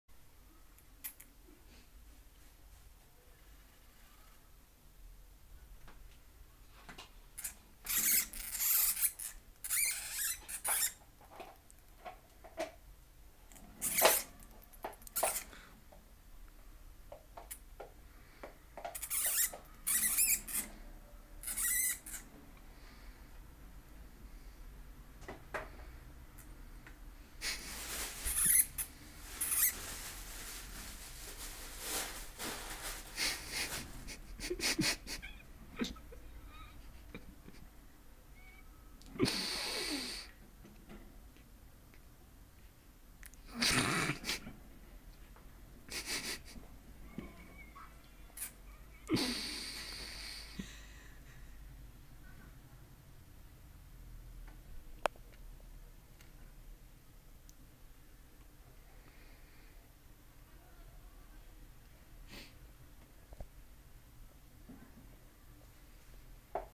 Два маленьких хомячки Жужа і Жуля не поділили клітку )
Навіть між хом'ячками бувають війни!
Ніби такі малесенькі , милесенькі , а насправді такі жорстокі і навіть кумедні )